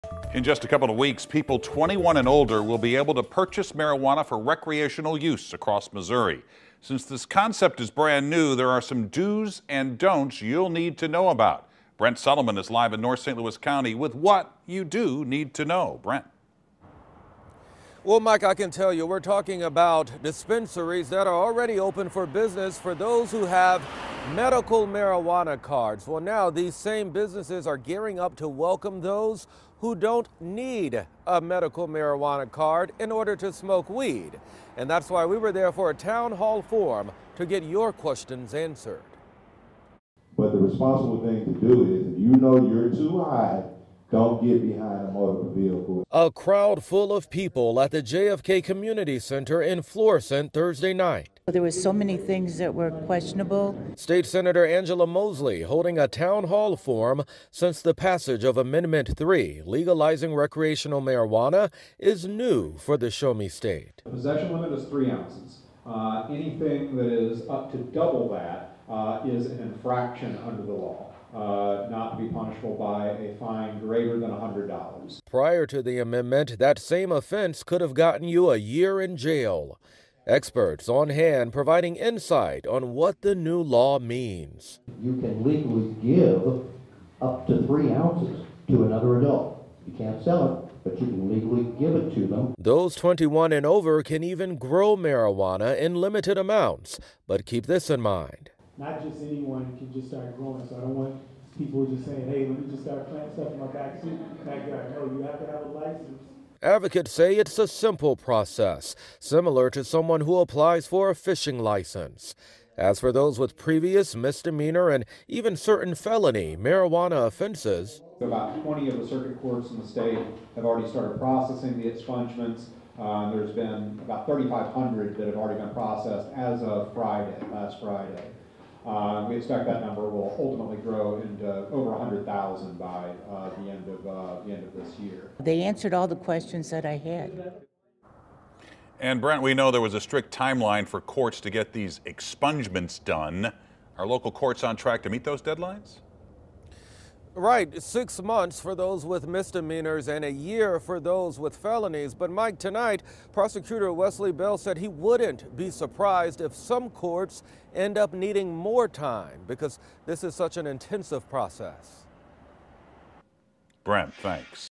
Crowds attend Florissant town hall forum addressing recreational marijuana
In just a couple of weeks, people 21 and older will be able to use recreational marijuana. Our Brent Solomon went to a town hall forum addressing questions.